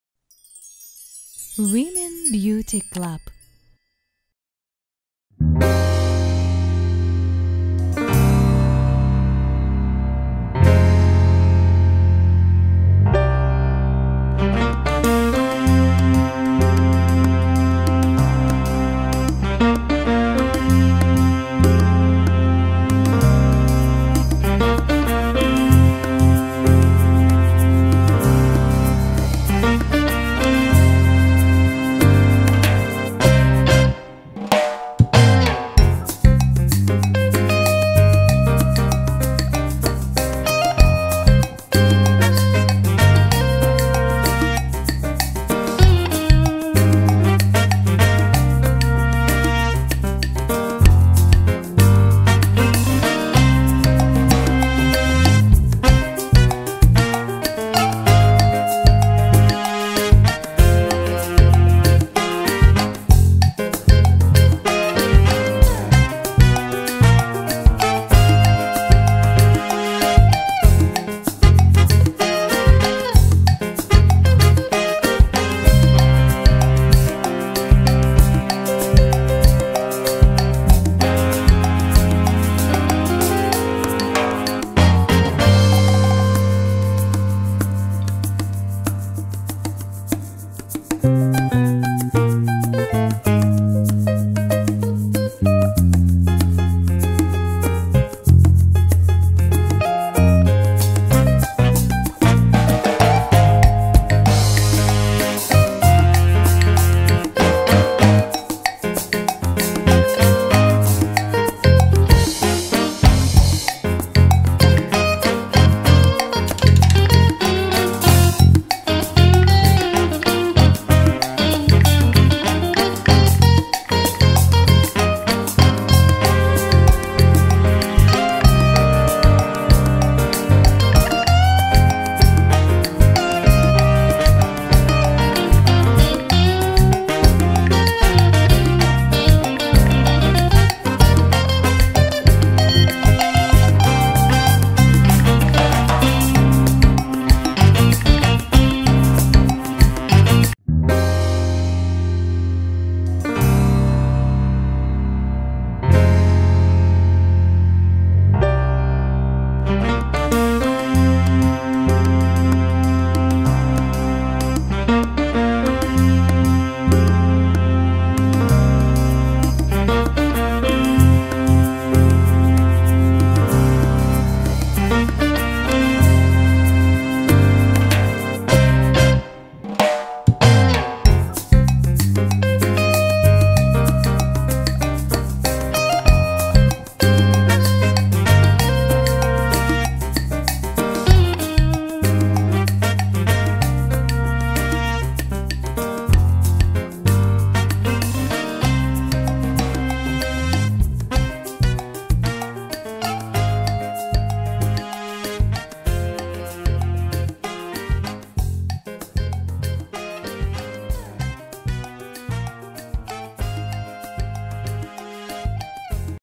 🔴 The best 3 fashionable shades of autumn ★ Women Beauty Club
I present to your attention, the best shades of autumn 2022. These shades will really complement your stylish everyday looks, help you look fashionable and modern.